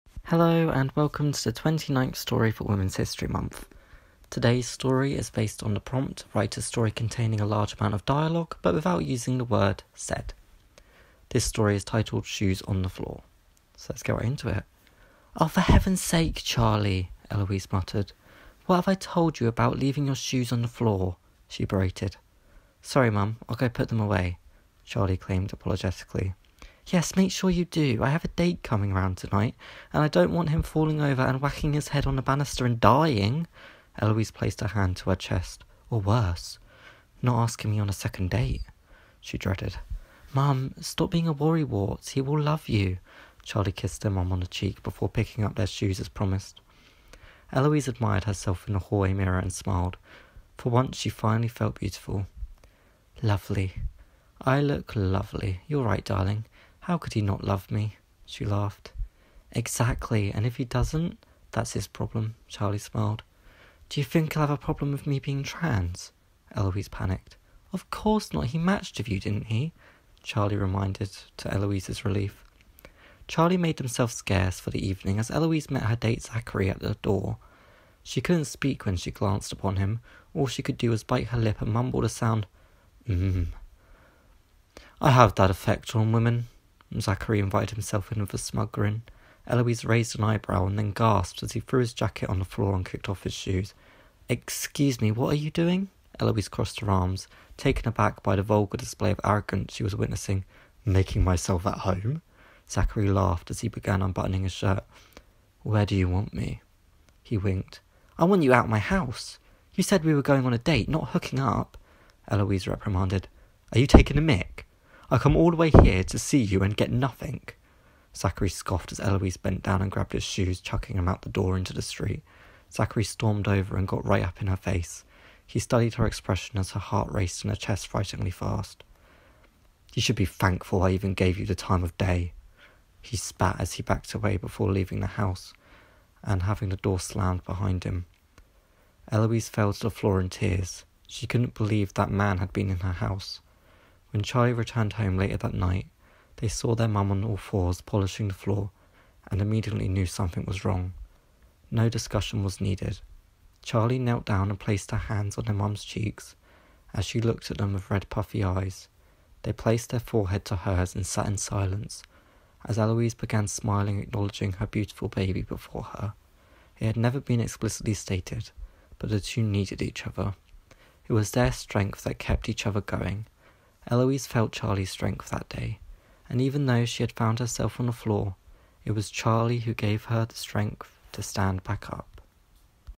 Shoes on the Floor | Prompt #126 (WHM #29)
This story contains references to transphobia, fear, misogyny, and sexual harassment so please continue at your own discretion.